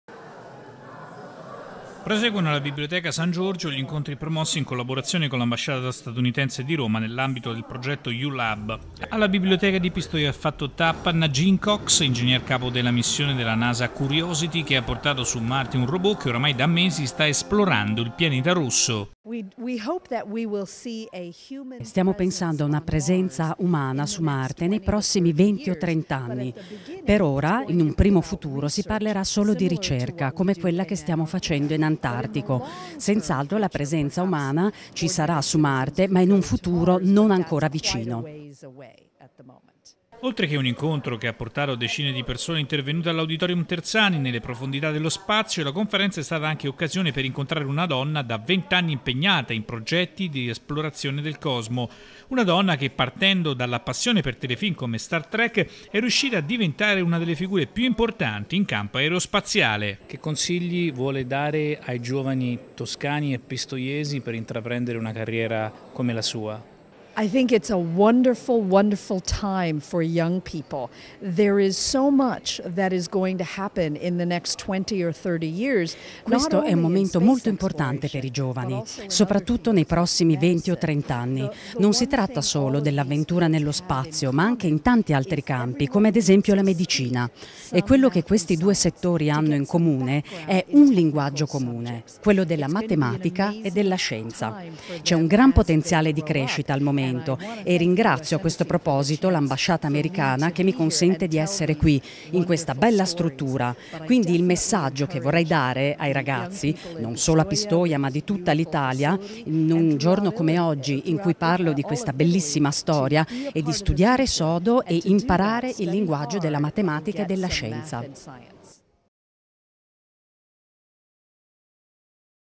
INGEGNERE NASA SAN GIORGIO
Nagin Cox, ingegnere capo della Nasa Curiosity in un incontro alla biblioteca San Giorgio di Pistoia. L'iniziativa si è tenuta nell'ambito di un rapporto di collaborazione avviato con l'ambasciata Usa a Roma. La Cox è stata responsabile del progetto che ha portato su Marte un robot per l'esplorazione del pianeta rosso.